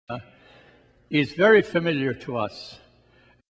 Second Sunday in Ordinary Time, January 20, 2019, 9:30 Mass
Homilist: Msgr. Connolly, Gospel: John 2:1-11

The ritual of love is far superior the ritual of law,